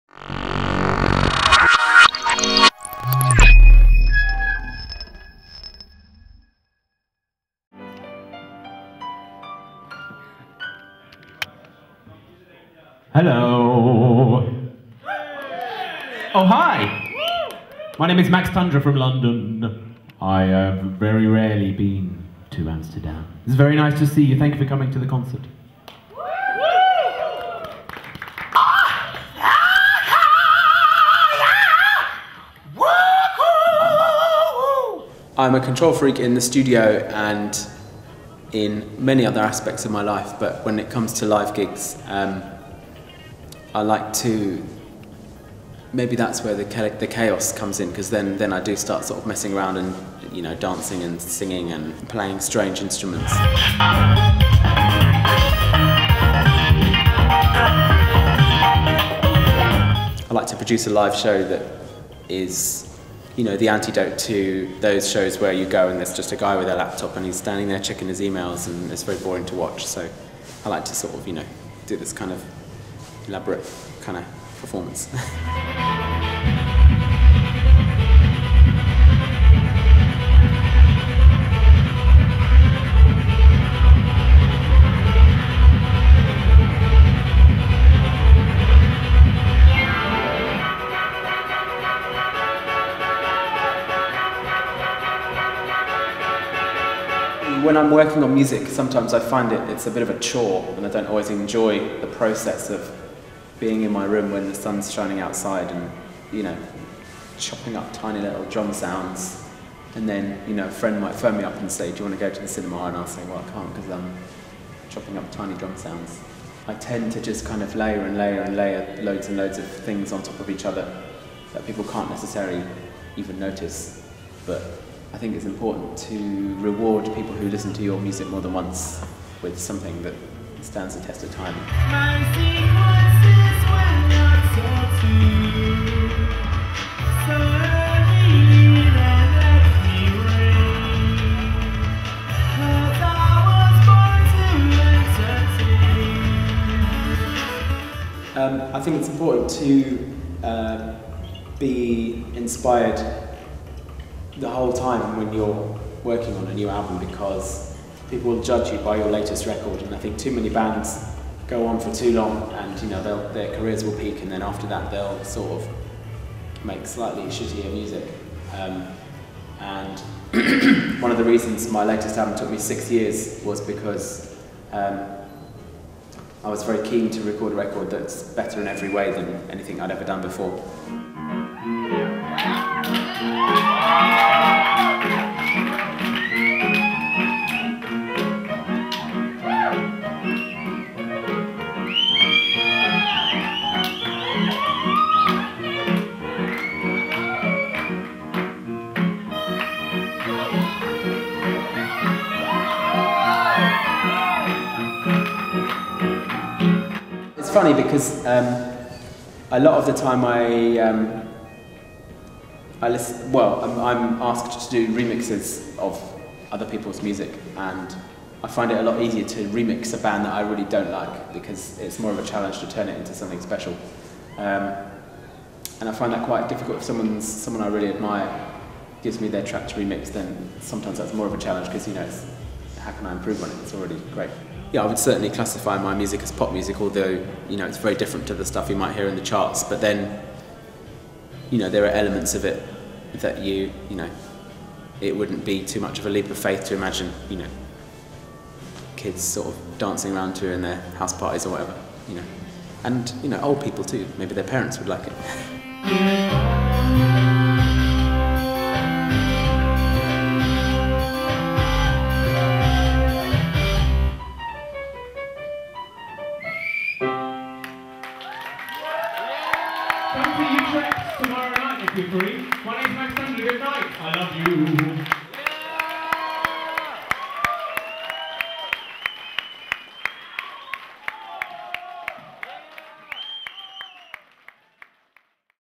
Max Tundra: twisted beats and toy pianos.
Max Tundra concert + interview @ Smart Project Space Amsterdam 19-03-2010